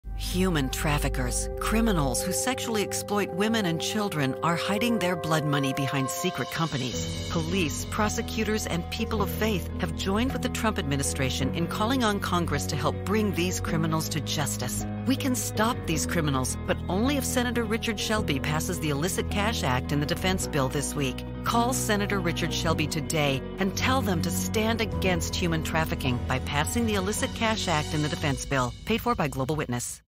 Sen. Shelby: Stand against human trafficking. Pass ILLICIT CASH.
Global Witness ran an multi-state ad campaign urging senators to include the ILLICIT CASH Act (S. 2563) in the annual defense bill. The legislation would end the abuse of anonymous shell companies and block human traffickers from financing their illicit activities. The ad aired over several days on Christian radio stations in Alabama.

Human traffickers take advantage of opaque corporate structures to mask their identities from law enforcement and launder their illicit profits.